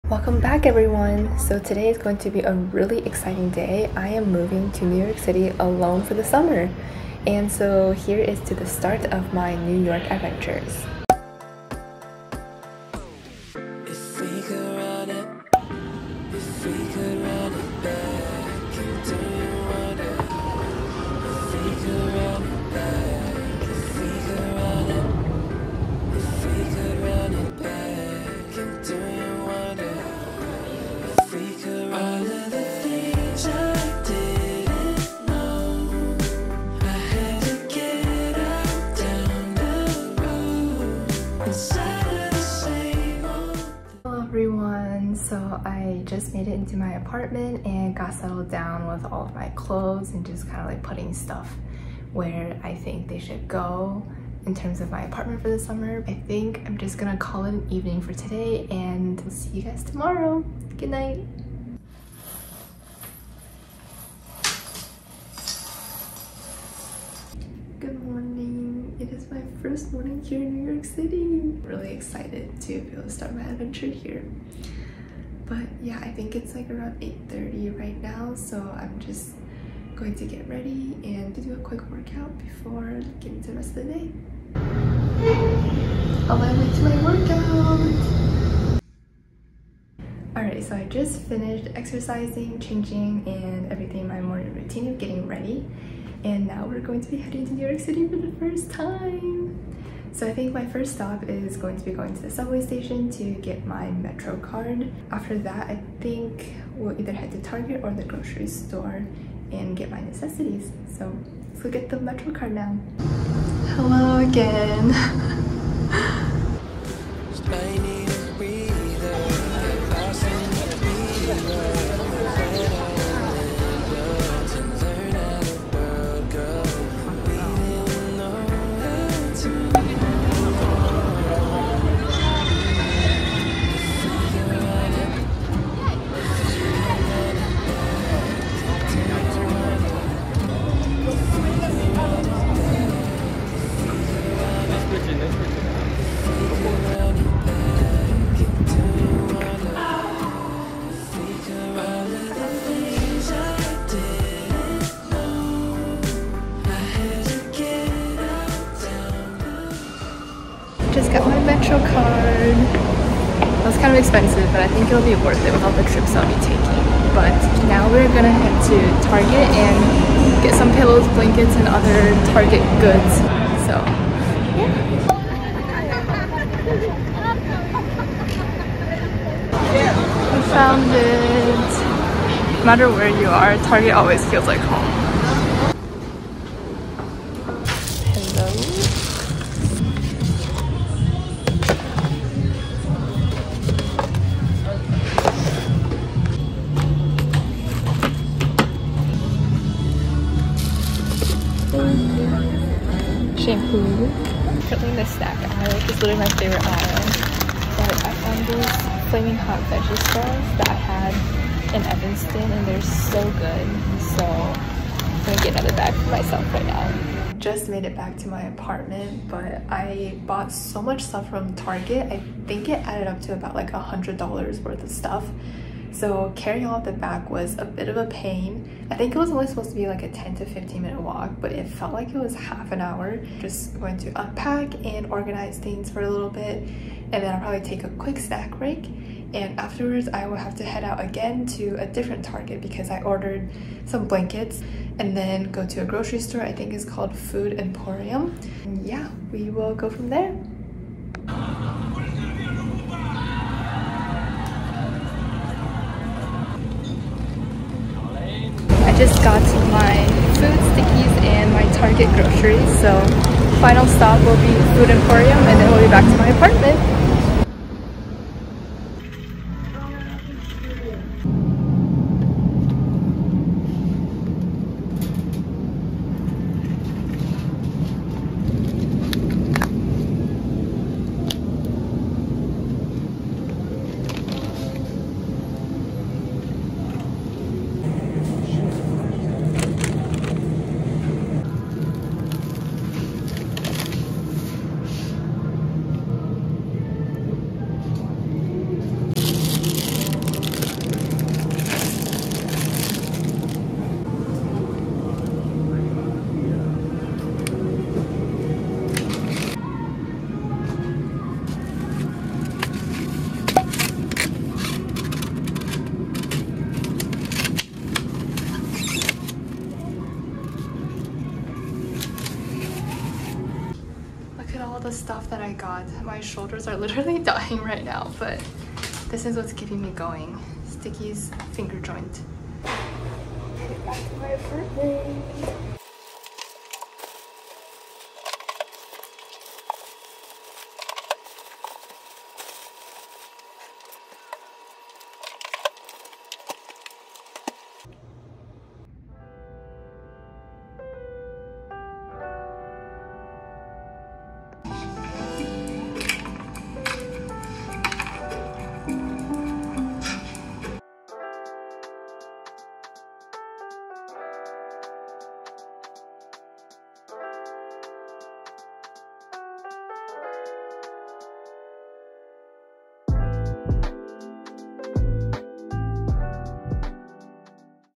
Moving to NYC ALONE 🗽| nyc diaries | moving in, times square, shopping, target runs, nyc sticky's!
I moved to NYC alone for the summer!!

Come with me as I move in, walk through Times Square (multiple times), do A LOT of shopping and Target runs, and try NYC's acclaimed chicken finger restaurant, Sticky's!!